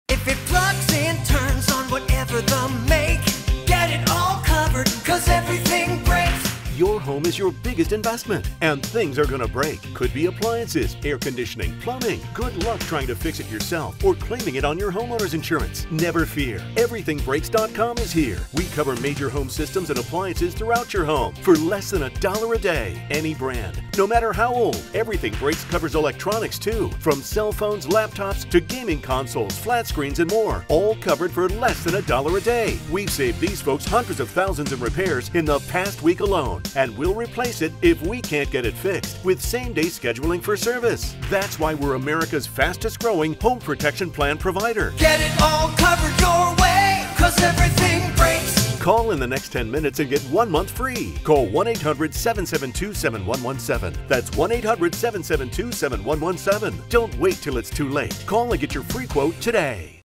Get It Covered
I did the motion graphics and editing for a home warranty company.